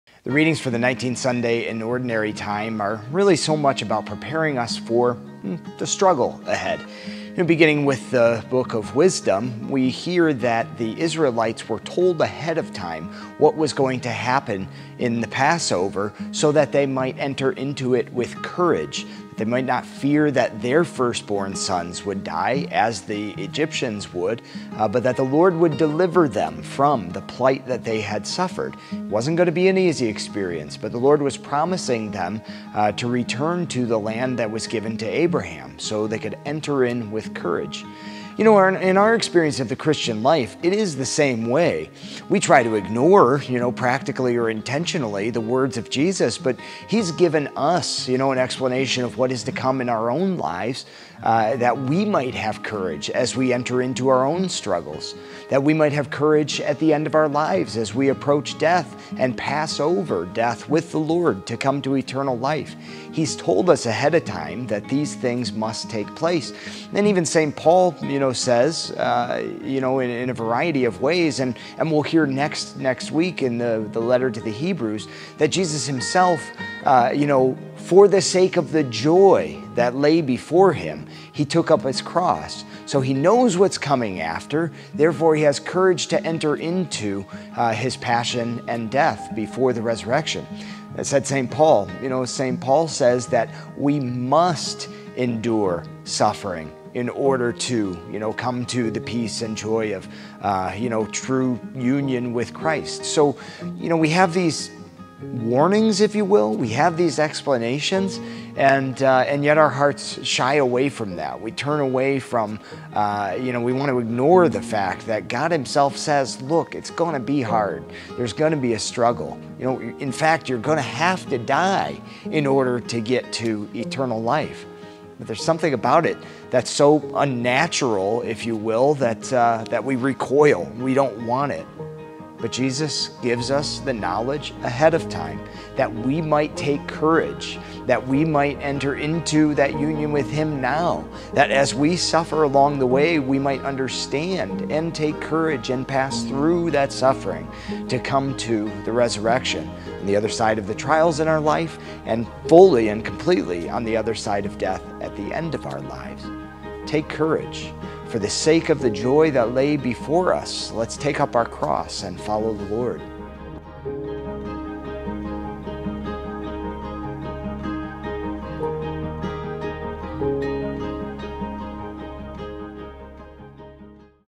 Sitting with Scripture with Fr. Andrew ~ PREPARING FOR THE STRUGGLE
PREPARING FOR THE STRUGGLE

God often prepares us for struggles or hard times ahead of them so that we might enter into them with courage, hope, and faith. The primary example? Our journey here on Earth to the end of our lives to eternal life with God. So let us take joy in what comes after our suffering and struggle, that there is purpose in our pain and hardship.